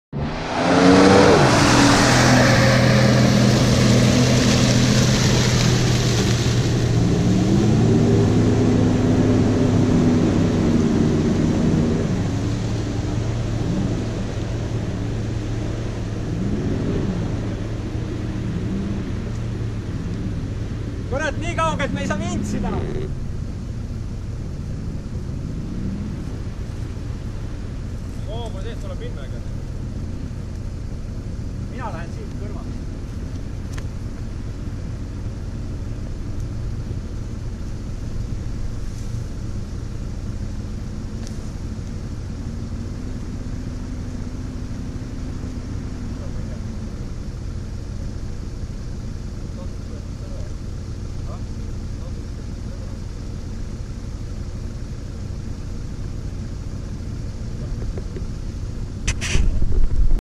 4x4 Off-Road Jeep Grand Cherokee ZJ V8 flat out in deep mud
Jeep zj 5.2 V8, 6,5“ lift and 35" Creepy Crawler MT.